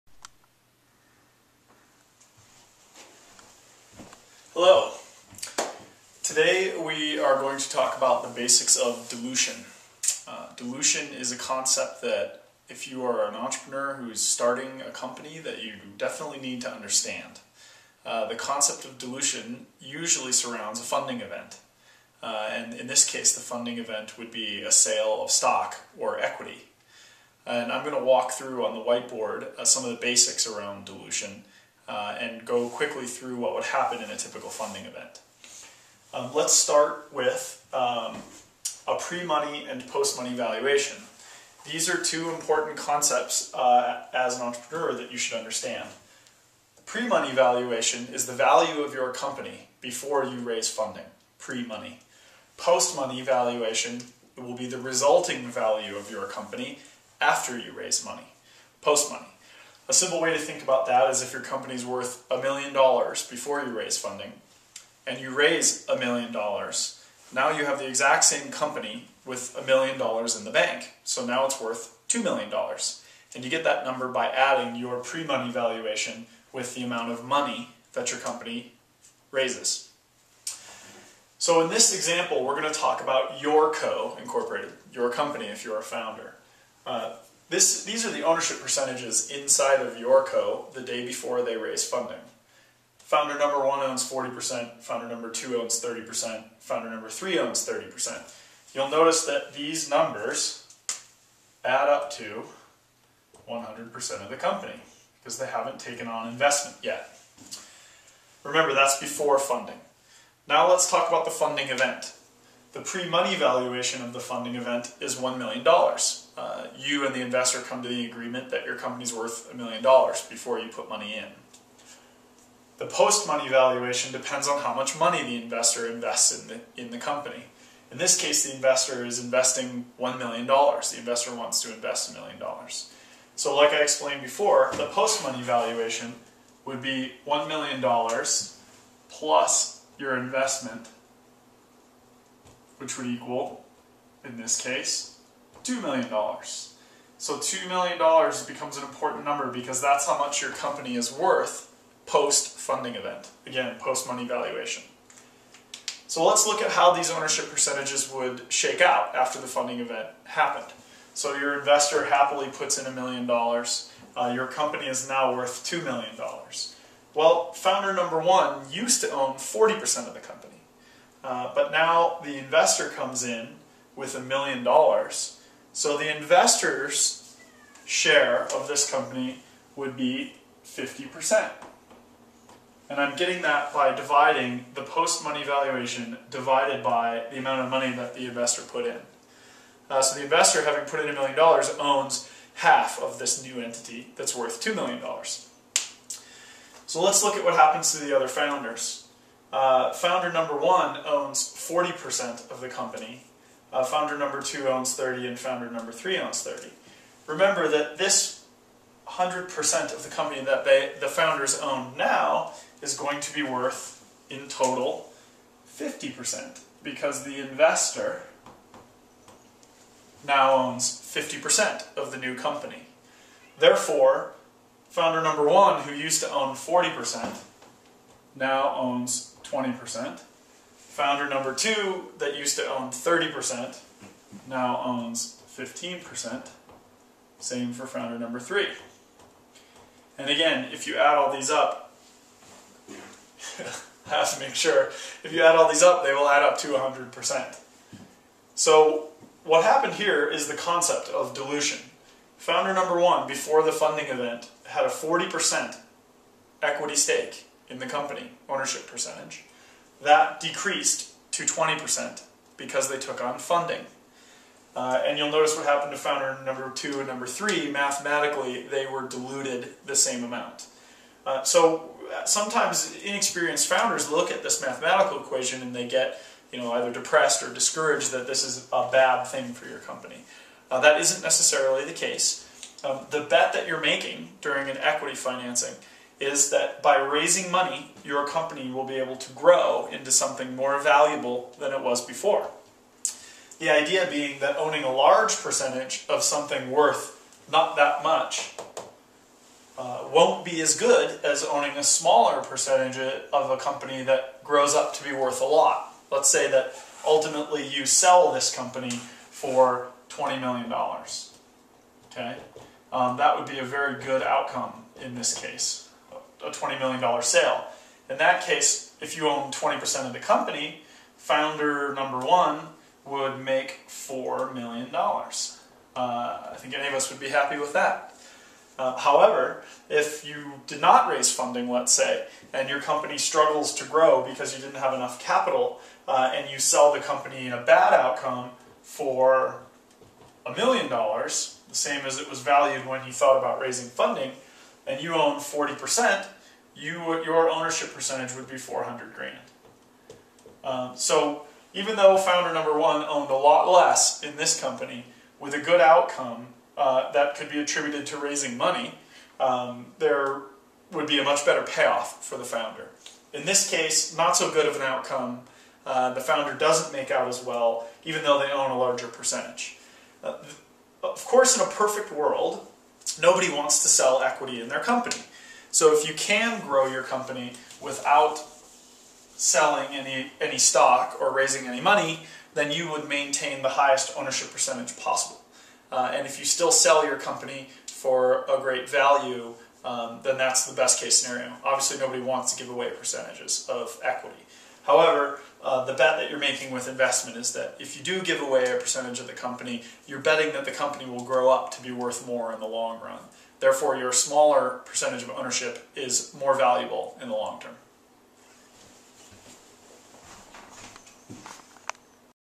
Dilution Basics
Dilution easily explained. What happens to my share ownership percentage when I raise money? What is the math behind the process? When does that lead to a good outcome and when is dilution bad?